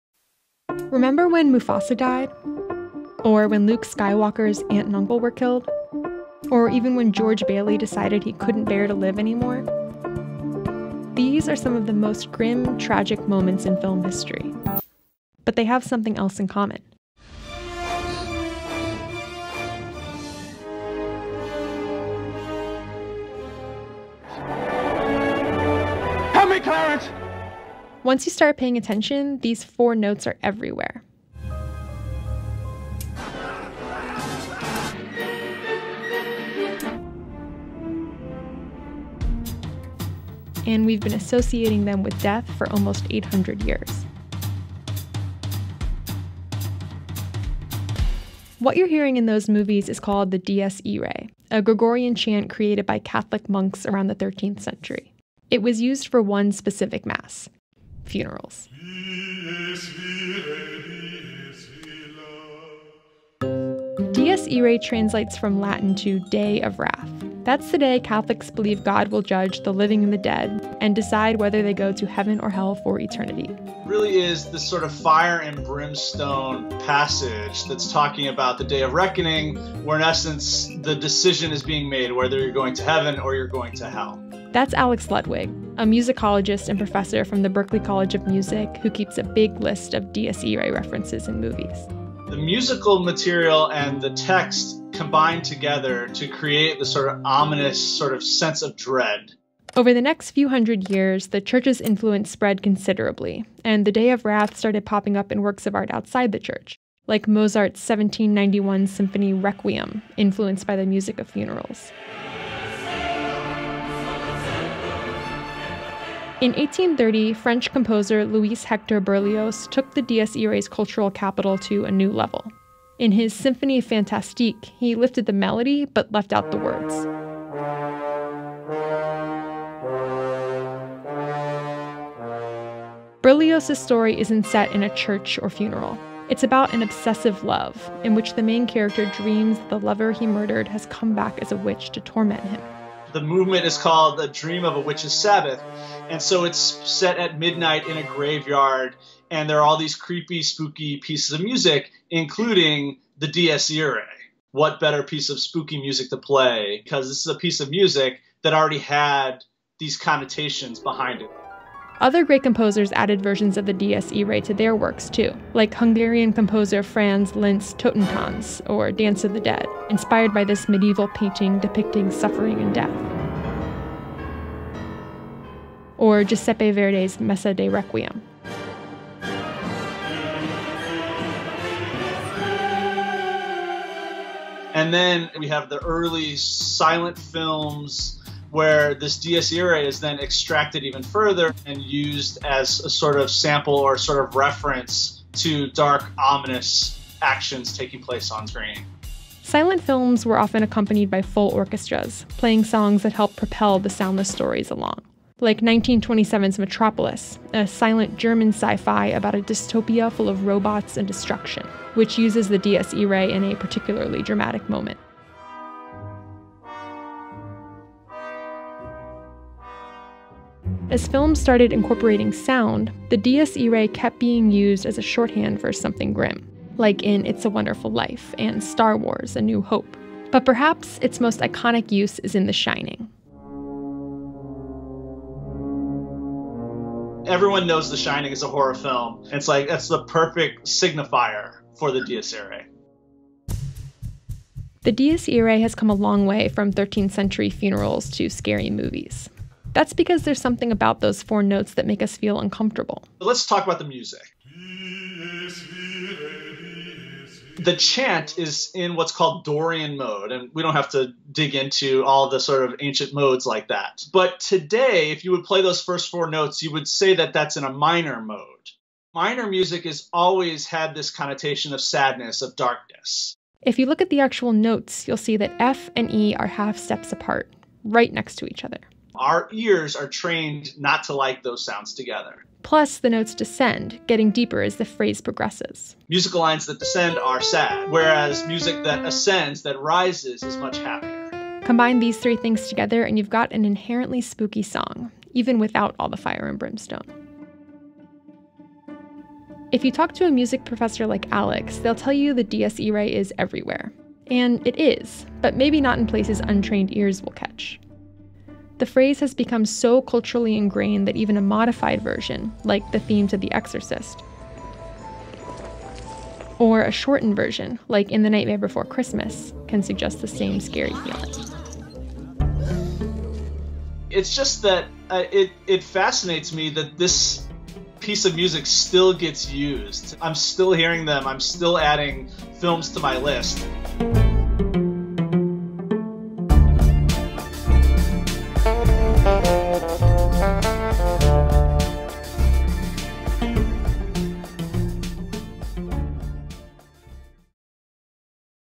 Why This Creepy Melody Is In So Many Movies
Is this a secret message in the form of musical code?